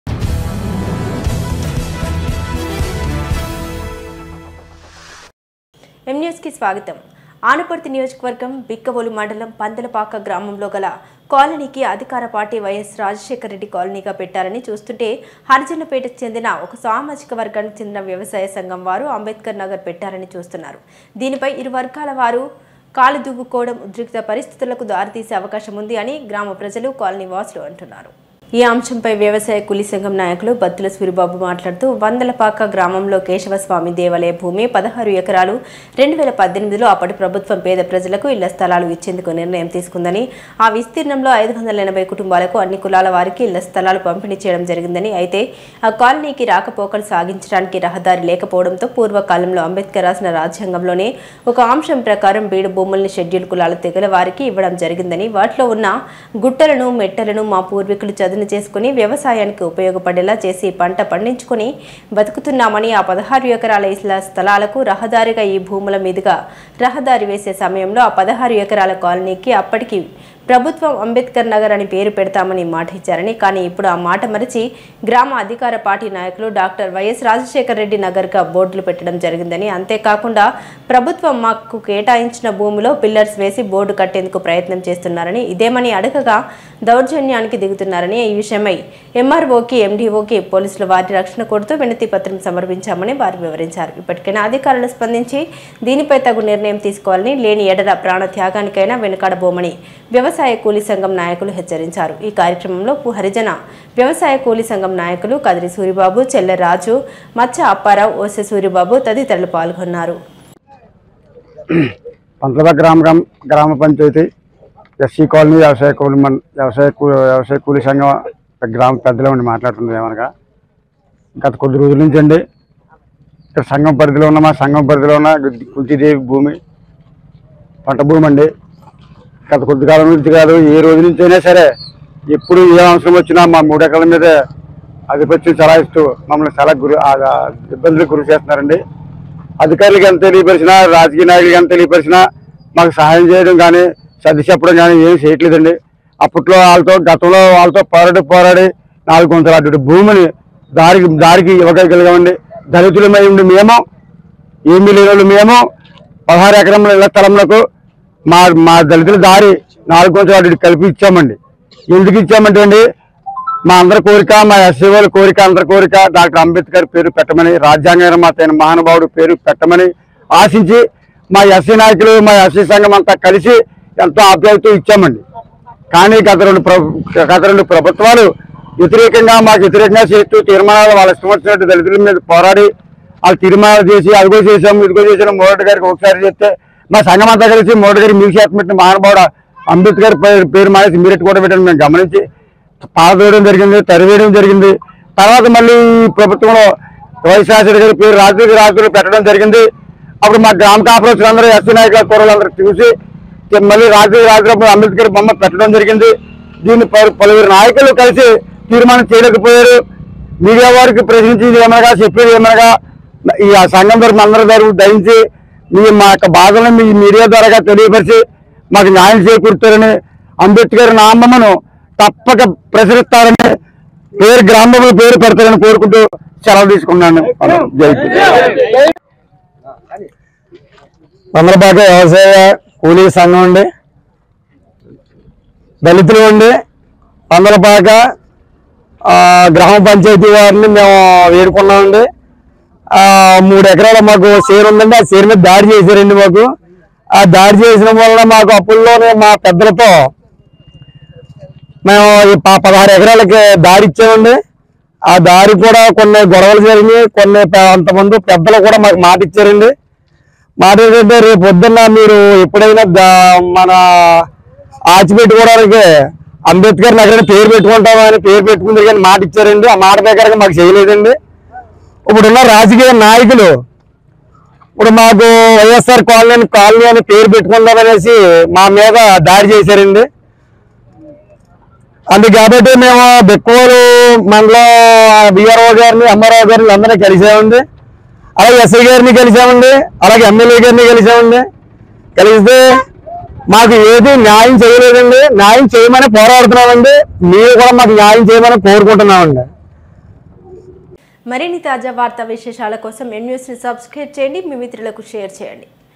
పందలపాక గ్రామంలో గల కాలనీకి అధికార పార్టీ వైయస్ రాజశేఖర్ రెడ్డి కాలనీగా పెట్టాలని వివాదం||M NEWS
అనపర్తి నియోజకవర్గం బిక్కవోలు మండలం పందలపాక గ్రామంలో గల కాలనీకి అధికార పార్టీ వైయస్ రాజశేఖర్ రెడ్డి కాలనీగా పెట్టాలని చూస్తుంటే హరిజన పేట చెందిన ఒక సామాజిక వర్గానికి చెందిన వ్యవసాయ సంఘం వారు అంబేద్కర్ నగర్ పెట్టాలని చూస్తున్నారు దీనిపై ఇరువర్గాల వారు కాలుదువ్వుకోవడం ఉద్రిక్త పరిస్థితులకు దారి తీసే అవకాశం ఉంది అని గ్రామ ప్రజలు కాలనీ వాసులు అంటున్నారు .||M NEWS TELUGU